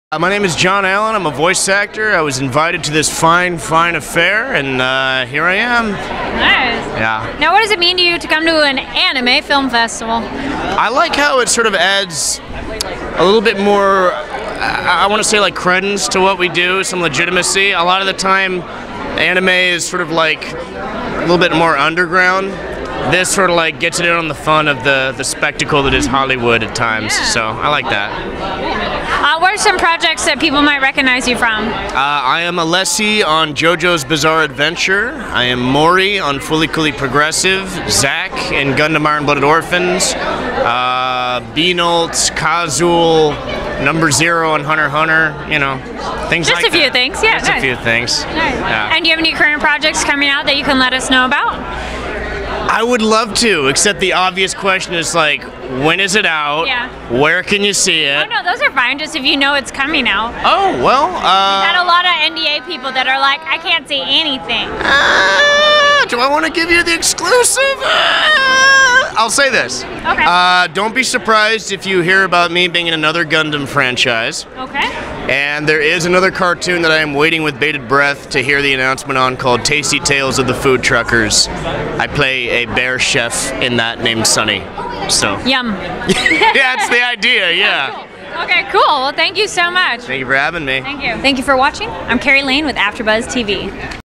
Jon Allen at Los Angeles Anime Film Festival
Host Kari Lane interviews Jon Allen (@KingJonAllen) at the red carpet for the Los Angeles Anime Film Festival.